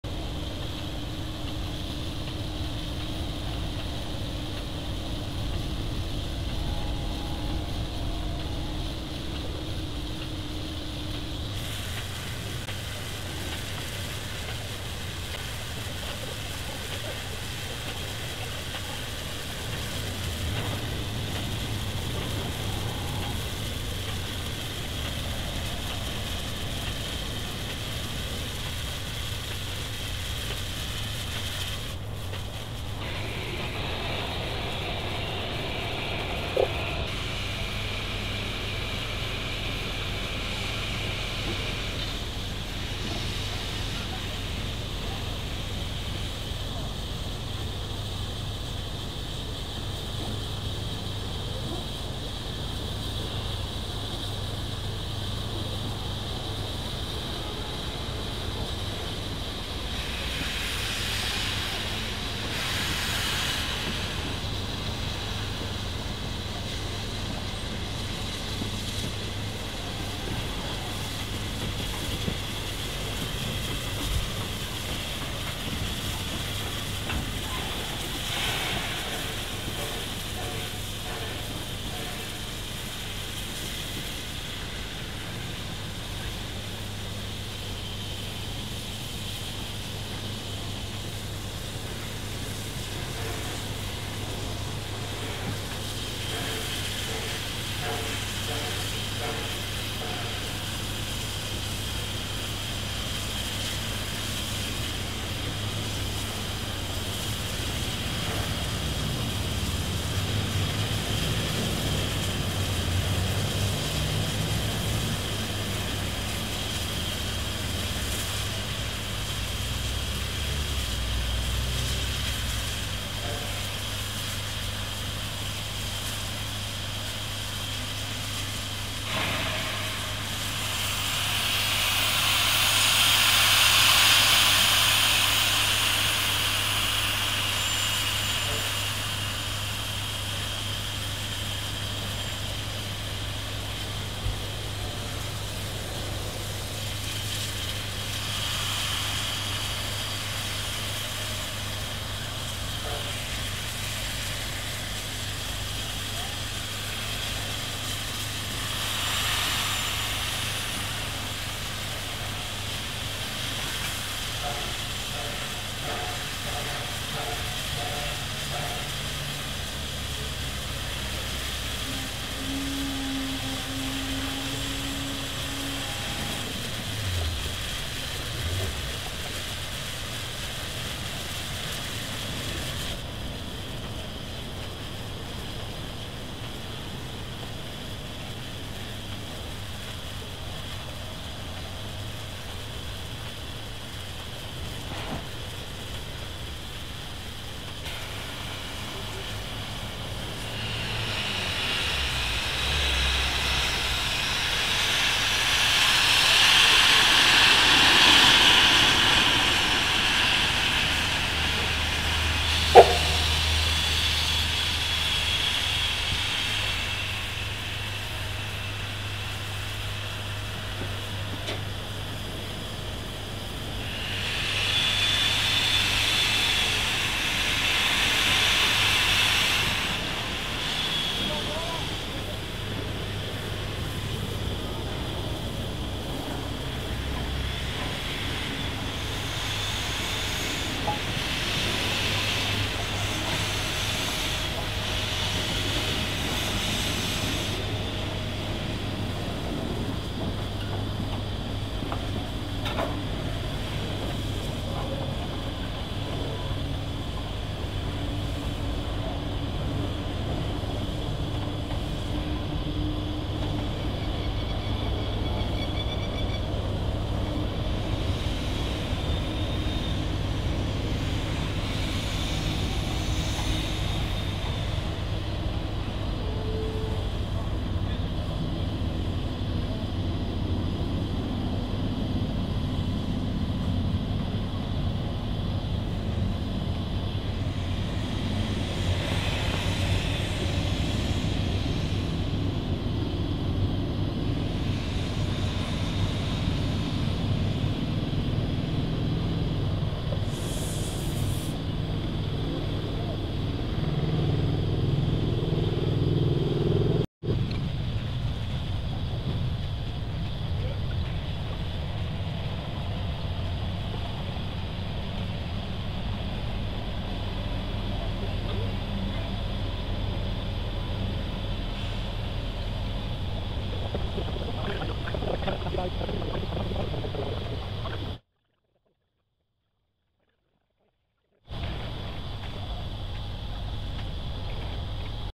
NSWGR 6029 and NSWGR 3526 Servicing After Transferring To Maitland For Steamfest
The Maitland Steamfest ) was held on the weekend of 13-14Apr2024. To get the trains there ready for an early morning start on Saturday, Transport Heritage NSW ) transferred NSWGR 6029 and 3526 their trains on the Friday. In earlier footage, provided by others, we saw NSWGR 6029 and 3526 in an historic parallel run at

The last two videos were taken by me of the arrival of NSWGR 6029 ) followed 10 min later by that of NSWGR 3526

This last video is of both locomotives being put to bed, which takes a long time, much longer than the over 18 minutes of the original footage. As such portions of this video have been sped up, a lot!

Also in this video in the background we see a Down Hunter Railcar, a triple headed One Rail (Genesis & Wyoming Orange and Black livery) empty coal train and Aurizon locomotive 3221 operating light towards Port Waratah.